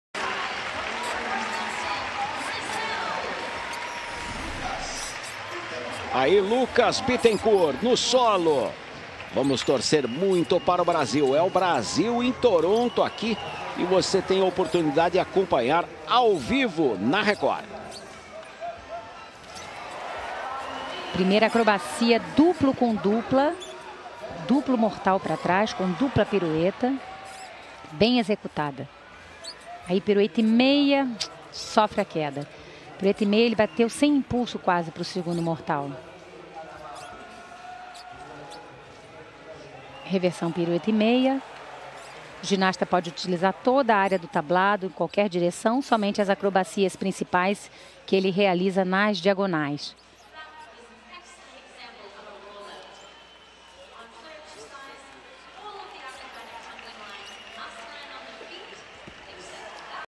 [HD] Veja a apresentação de Lucas Bitencourt no solo da Ginástica Artística - Pan Toronto 2015
Jogos Pan-Americanos de Toronto 2015
13/07/2015
Ginástica Artística Masculina